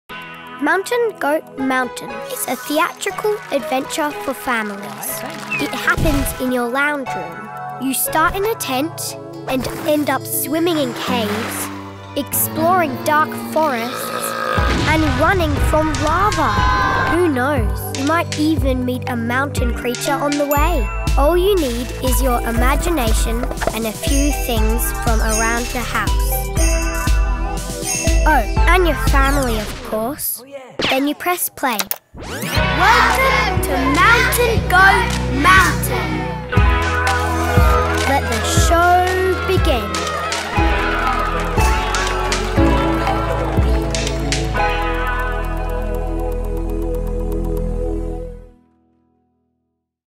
Welcome to Mountain Goat Mountain!
Get your family together for a fabulous lounge room adventure!

"The children have asked to do 'Mountain Goat Mountain' again and again…"

"An immersive online experience... perfectly pitched."
— Sunday Business Post

"One of the most beautiful family arts adventures I’ve ever seen."
— Early Arts UK

For more information about Mountain Goat Mountain or to purchase a pass

Created from the drawings of families from all around the world - these unique mountain creatures have been brought to life by animator Chris Edser with music by Zoë Barry and Jed Palmer.